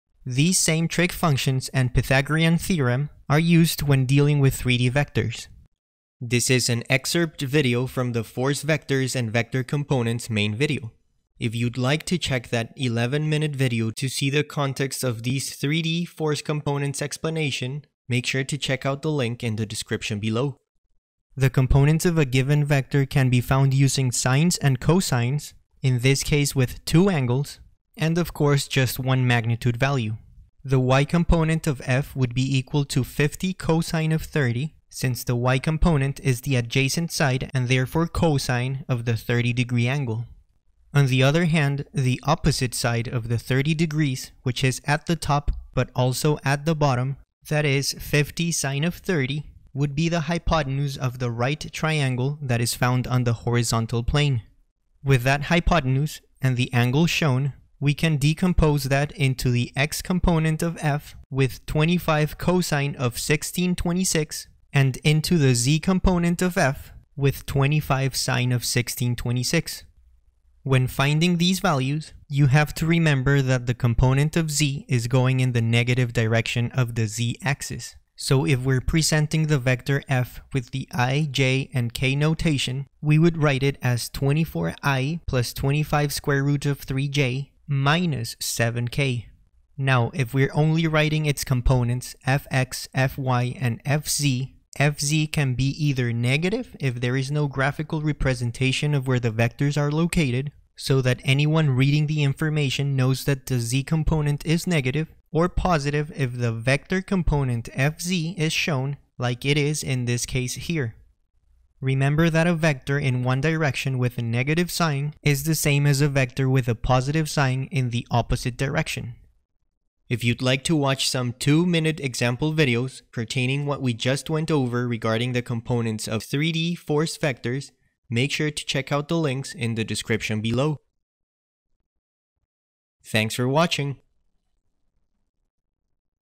3D VECTOR Components in 2 Minutes! - Statics
Finding components of a 3D vector using its magnitude and angle directions.

EXCERPT FROM:

Main Video:
Force Vectors and VECTOR COMPONENTS in 11 Minutes! - STATICS

Statics Course Links:
WATCH ALL STATICS CONTENT IN LESS THAN 2 HOURS!